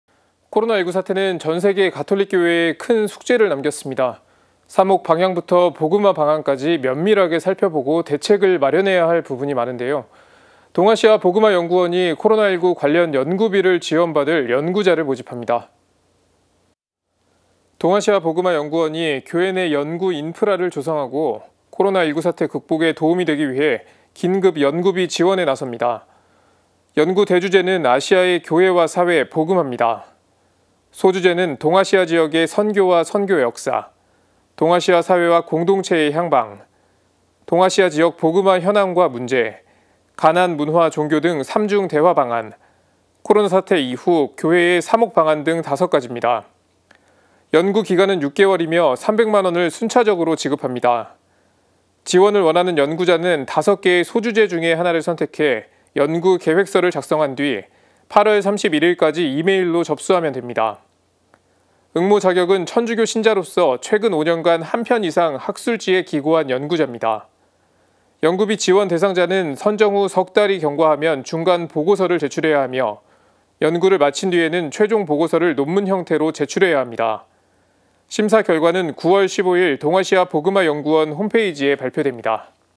동아시아복음화연구원 "코로나 연구비 지원합니다"
코로나19 사태는 전 세계 가톨릭교회에 큰 숙제를 남겼습니다.
사목 방향부터 복음화 방안까지 면밀하게 살펴보고 대책을 마련해야 할 부분이 많은데요.
동아시아복음화연구원이 코로나19 관련 연구비를 지원받을 연구자를 모집합니다.

카카오톡 플러스친구 : cpbc TV